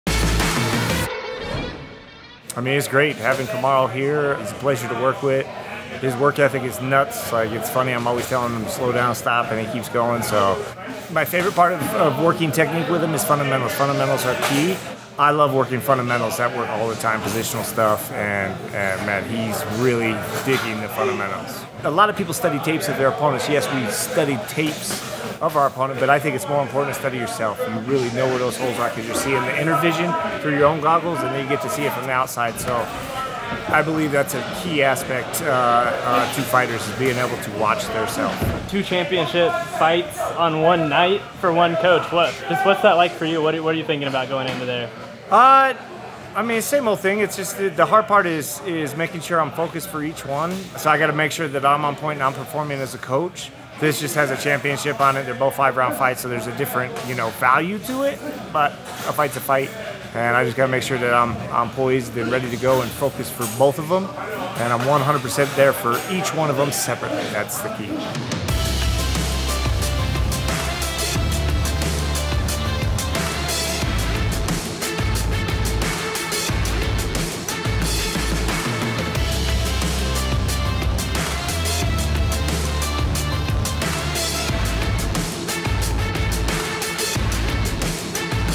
TWO TITLE FIGHTS in ONE NIGHT! | Trevor Wittman on Coaching Kamaru and Rose at UFC261
I caught up with Coach Trevor Wittman at Elevation Fight Team in Denver to talk about coaching Kamaru Usman and Rose Namajunas as they both fight for World Championships on the same night at UFC 261!

Filmed and edited by Kidron Cannon.

The gear used was Canon c200.